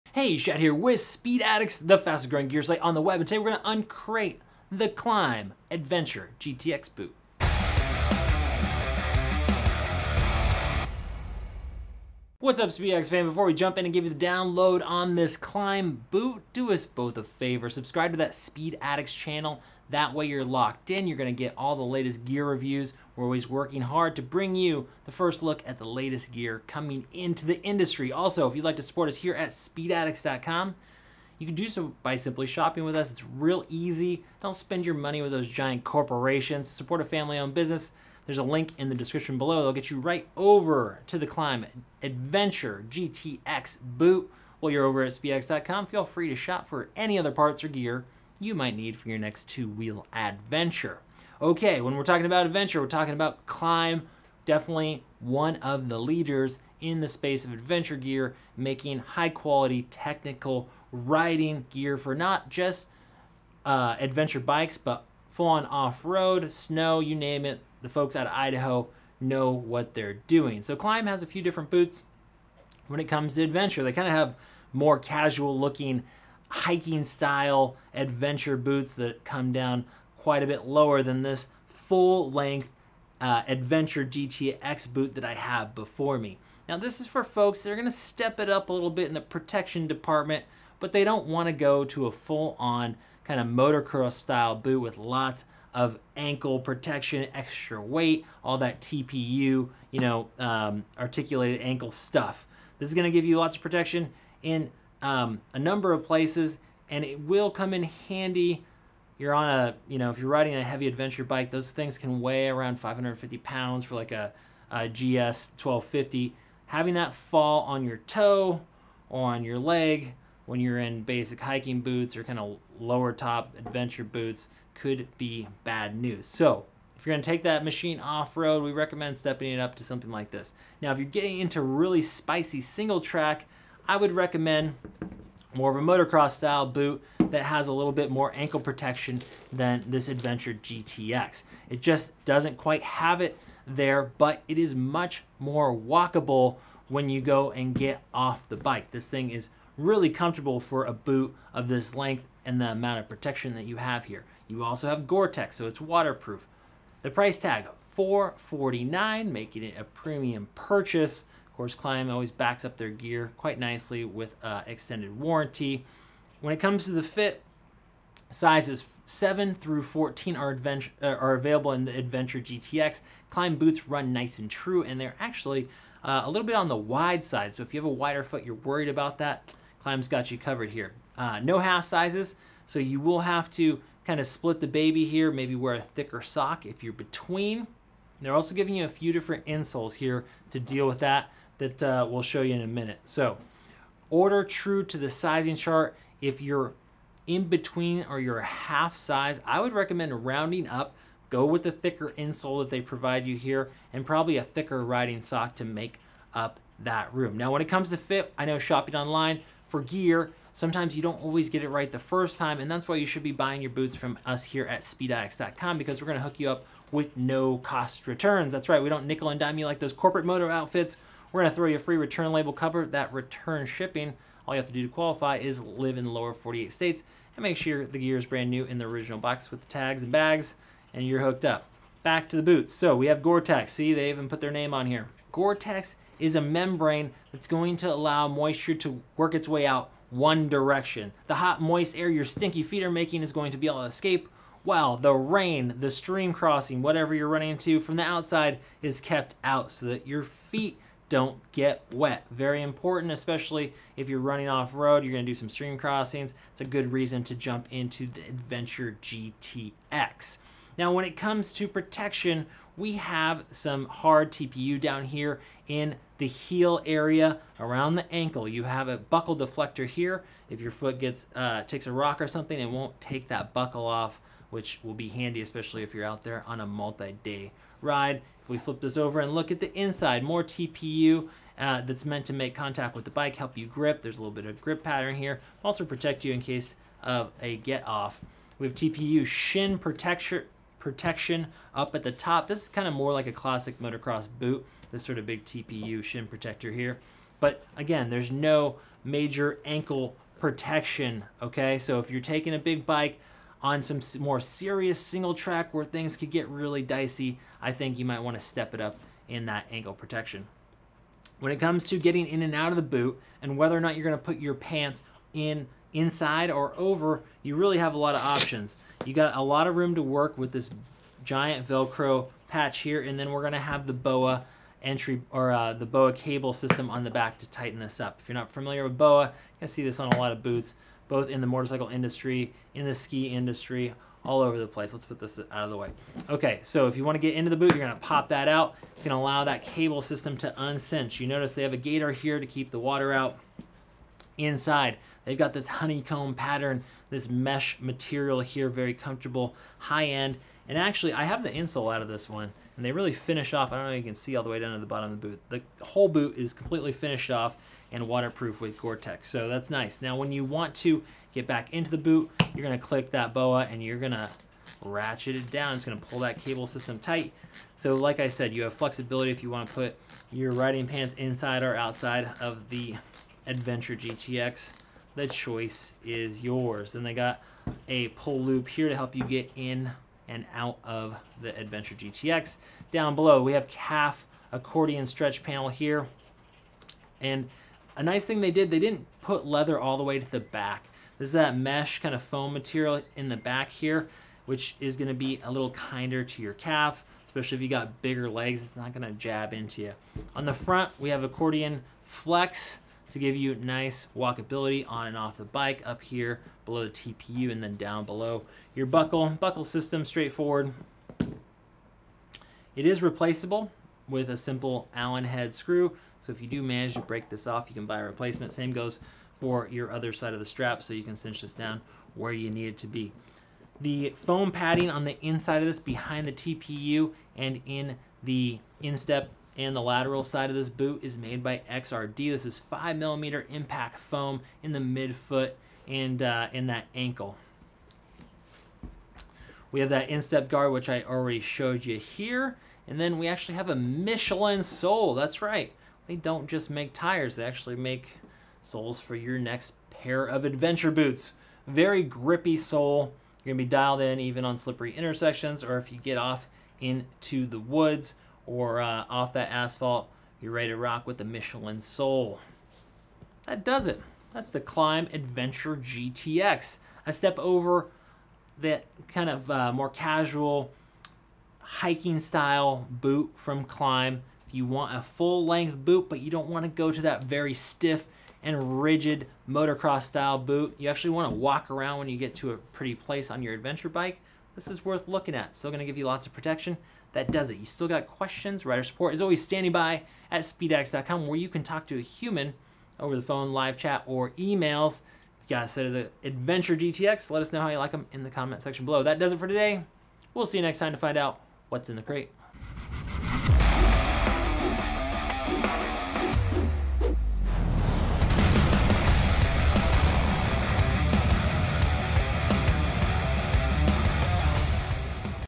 Klim Adventure GTX Boots Review at SpeedAddicts.com
KLIM ADVENTURE GTX BOOTS - AVAILABLE NOW AT SPEEDADDICTS.COM

Adventure touring requires a fine balance between riding protection, weather protection and walking comfort. The Adventure GTX boots are engineered from rugged materials, using an intuitive design to bring comfort, durability and ease of use to any adventure touring ride. Unique feature sets deliver unrestrictive support tailored for long distances, without eliminating important walking comfort for visiting your destination.